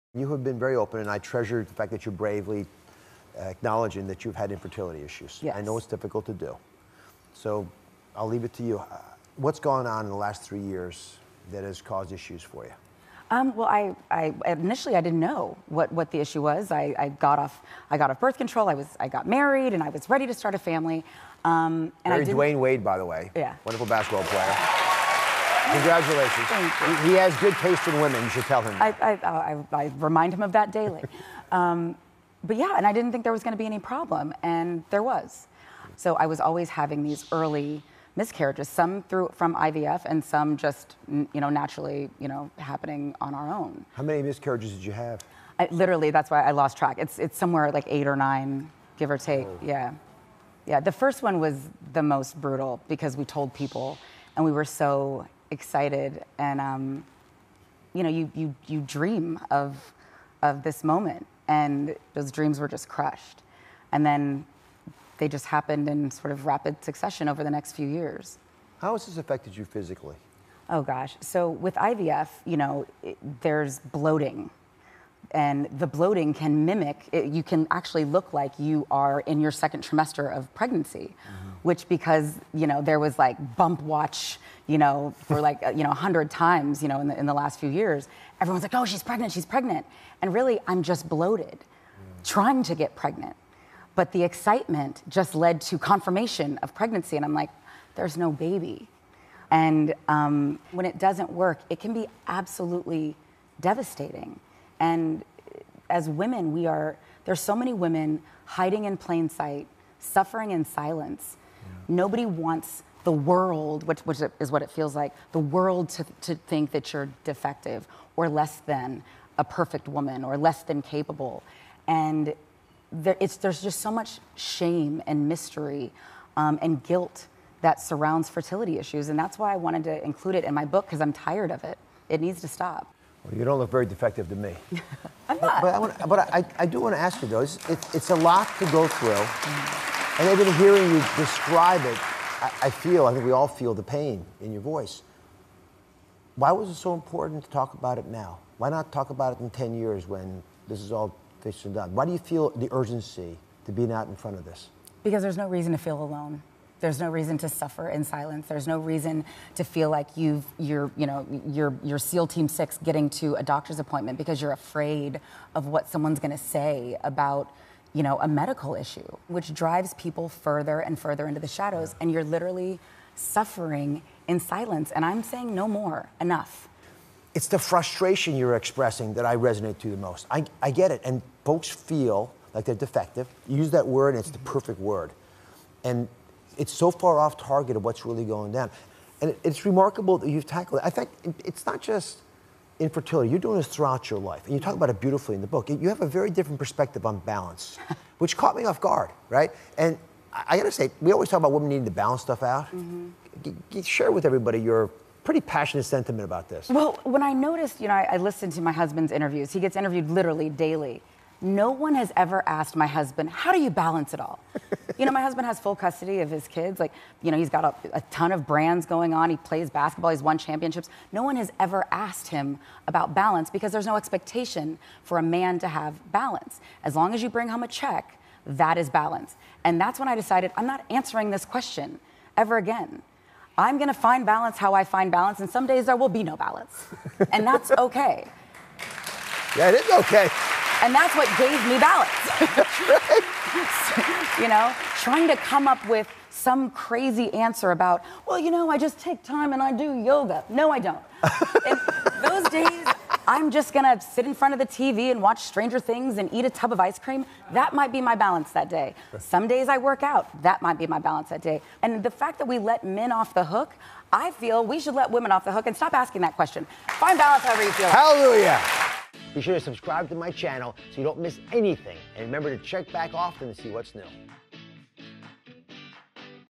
Gabrielle Union opens up about Fertility issues
Women's health.